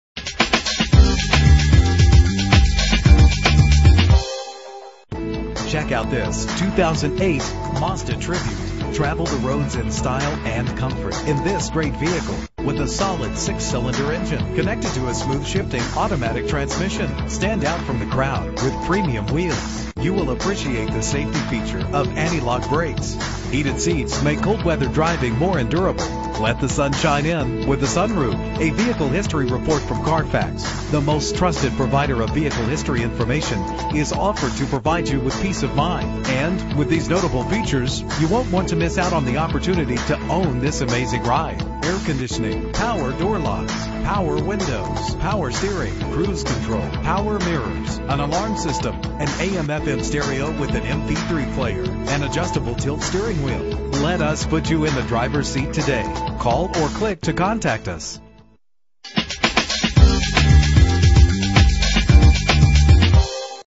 2008 Mazda Tribute - Gladstone OR
Year: 2008
Make: Mazda
Model: Tribute
Trim: s Sport
Engine: 3.0 liter 6 cylinder 24 valve
Transmission: 4-Speed Automatic
Color: Black
Mileage: 50148
Address:
19400 SE McLoughlin Blvd